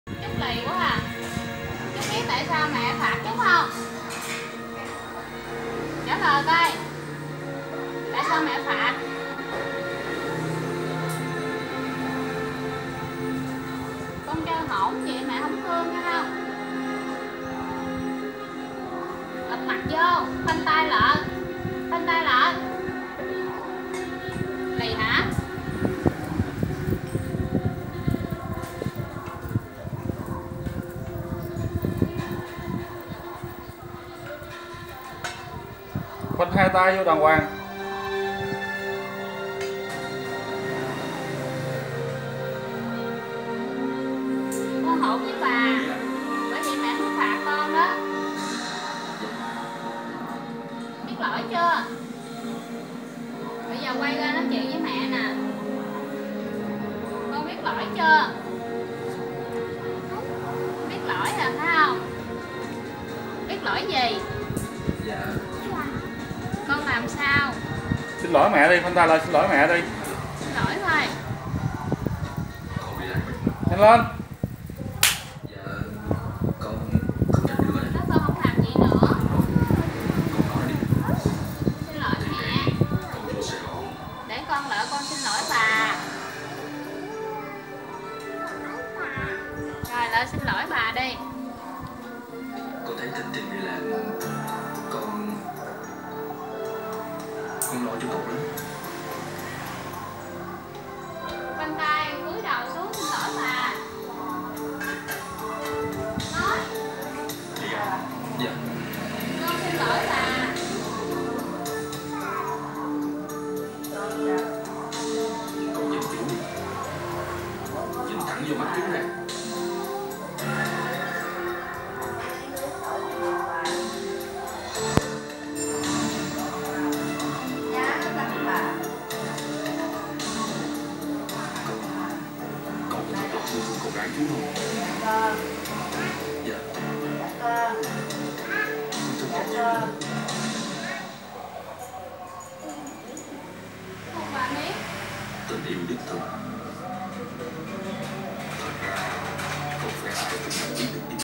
Trúc ỉa lì bị mẹ phạt. Bé Na Thanh Trúc càng lớn càng quậy phá bị mẹ phạt hoài.
Xem clip và ủng hộ kênh bằng cách đăng ký và bấm nút chuông 🛎để nhận video mới nhất!!! ❤❤❤ Thanks 😍😍😍